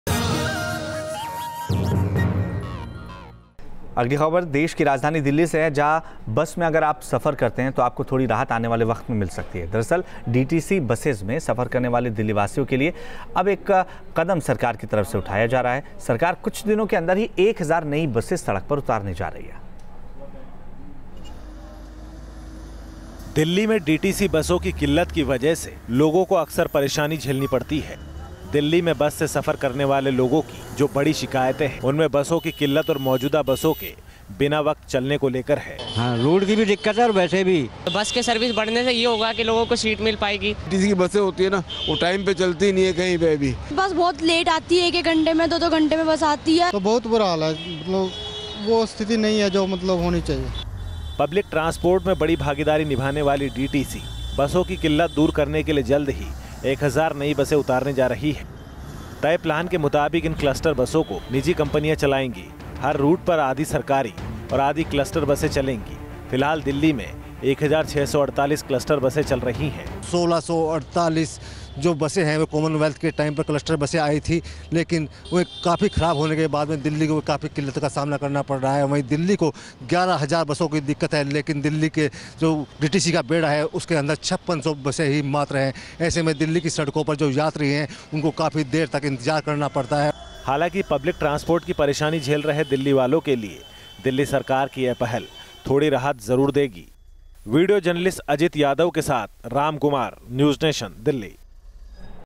Delhi Government announces to add 1000 new cluster buses in Delhi Transport Corporation
To remove the crisis of public transport, the Delhi Government announced to add 1000 new orange coloured cluster buses in Delhi Transport Corporation (DTC). However DTC need 11,000 buses but it has  5,600 buses only.